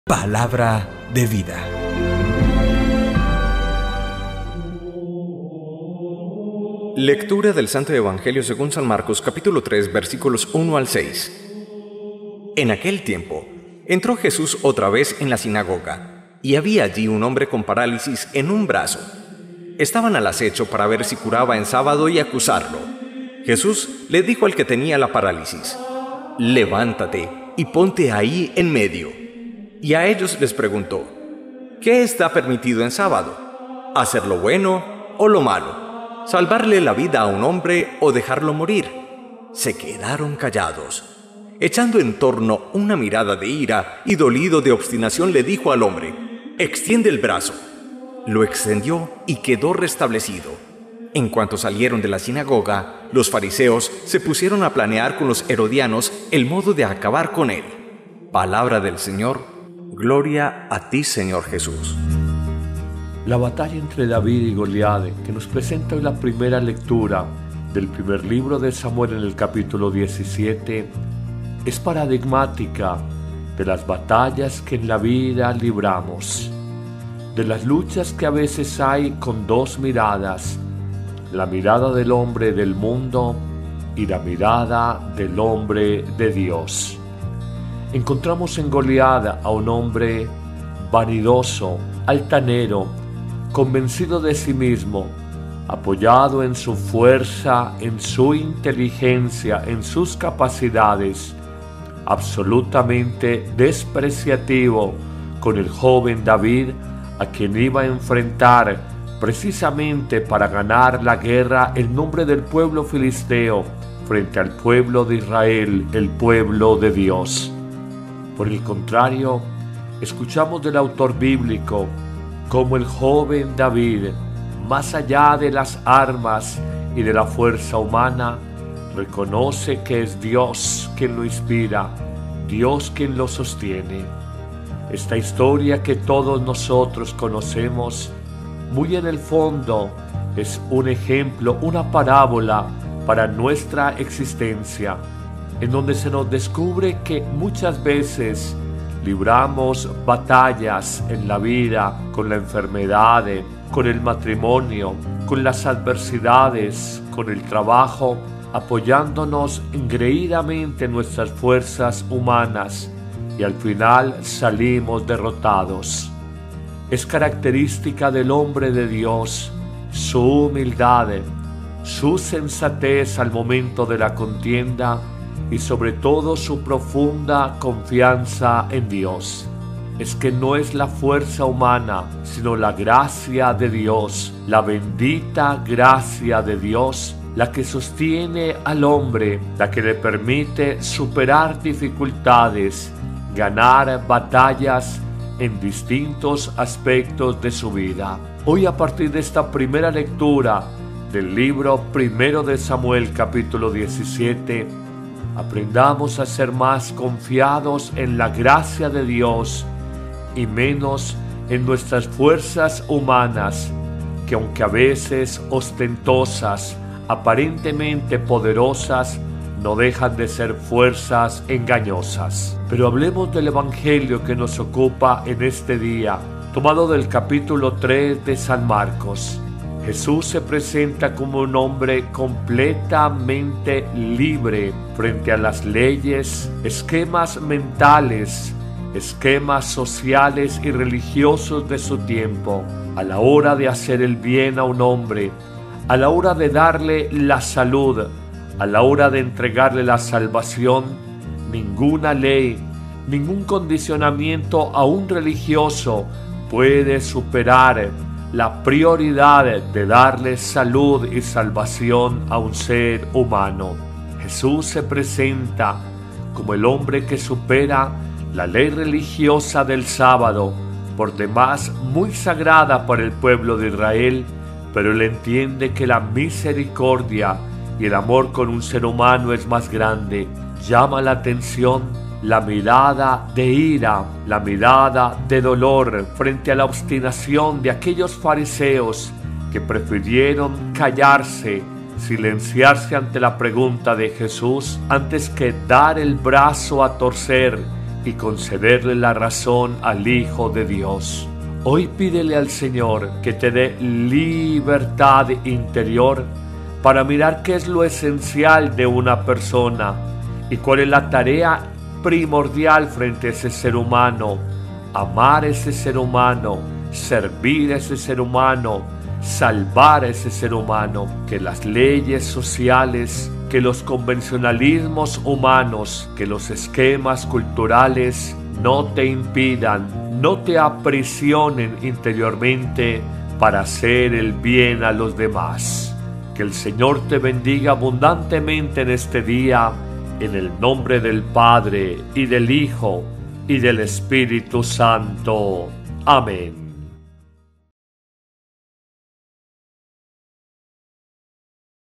Evangelio de Hoy Miércoles 17 de Enero 2018 | Padre Carlos Yepes
Vive el Evangelio de hoy. Únete con devoción para reflexionar este miércoles 17 de enero de 2018.

GANA BATALLAS DE VIDA Y VIVE CON LIBERTAD INTERIOR PARA HACER EL BIEN.

El evangelio de hoy miércoles 17 de enero de 2018 de San Marco 3, 1-6 y la primera lectura de Samuel 17 sobre David y Goliat nos muestran 2 enseñanzas de vida:

1. LAS BATALLAS DE LA VIDA SE GANAN CUANDO TENEMOS A DIOS.

¿QUE PAPEL DESEMPEÑAMOS EN NUESTRO DIARIO VIVIR?

¿Somos como David? o ¿Somos como Goliat?

- GOLIAT es el Hombre vanidoso, altanero, despreciativo, convencido de su capacidad, de su inteligencia, de su fuerza y poderío humano. Esta es la mirada del hombre del mundo.

- DAVID Es Joven que más allá de las armas y la fuerza humana, reconoce que es DIOS él que lo inspira y lo sostiene. Esta es la mirada del hombre de Dios. Muchas veces libramos batallas en la enfermedad, la adversidad, el matrimonio, el trabajo y nos apoyados engreídamente en nuestras propias fuerzas o las del ser humano, sin saber o sin reconocer que es la bendita gracia de DIOS la que nos permite ganar estas batallas y superar obstáculos.

Señor, ayúdanos a tener la mirada del hombre de DIOS.

2. LIBERTAD INTERIOR

JESÚS SE PRESENTA LIBRE POR ENCIMA A ESQUEMAS Y LEYES Y
HACE LO QUE DEBE HACER, CURAR EN UN DÍA SÁBADO.

La libertad interior implica tener claro varios aspectos:
- A la hora de hacer el BIEN ningún condicionamiento, ley social puede evitar que hagamos el BIEN.
- Miremos lo esencial de cada ser humano.
- Cumplamos nuestra tarea de amar, de servir y apoyar otras personas.

Señor, ayúdanos a ser libre interiormente para hacer el BIEN.

RENUEVA TU VIDA